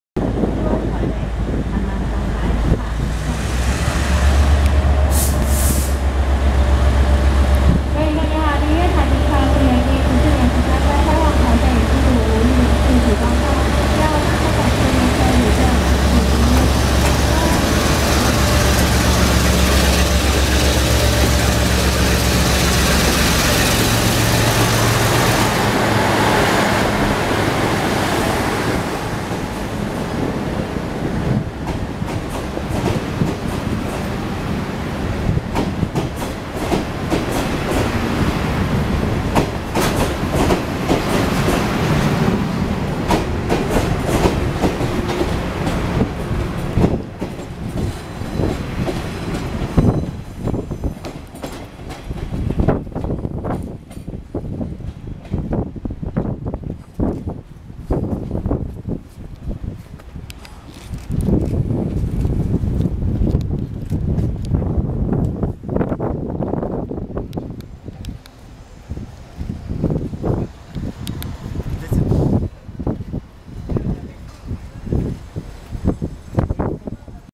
|交通迷天地| [台灣鐵路] 冷平車 南方公園專列 往內灣 6708次 新竹離站
拍攝車站: 新竹站
由新左營 開往內灣 6708次 專列
車況: 待發
相機型號:CASIO EX-ZR1200

Taiwan Railway
Record Date: 11/7/2020
Record Station: Hsinchu Station
Train NO: 6708
From Xinzuoying to Neiwan Station
(Air-conditioned Flat Special Train)